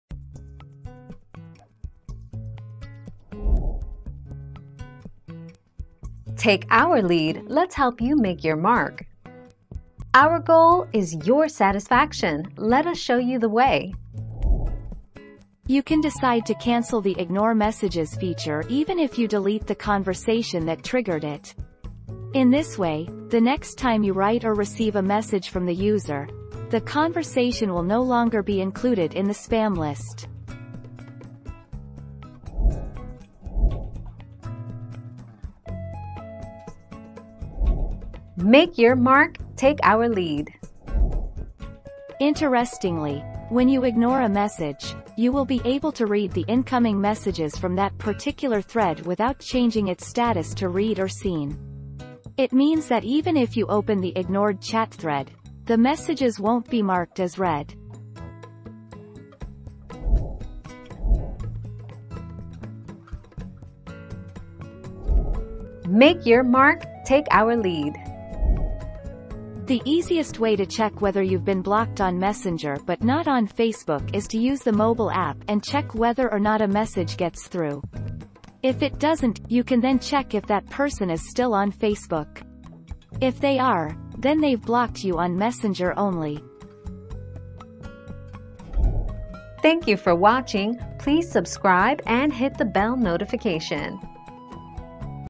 What happens if I delete an ignored message?
00:00 - What happens if I delete an ignored message?
00:34 - Can ignored messages be seen?
01:01 - How do you tell if you are blocked or ignored on messenger?

Laura S. Harris (2021, September 16.) What happens if I delete an ignored message?
    AskAbout.video/articles/What-happens-if-I-delete-an-ignored-message-262509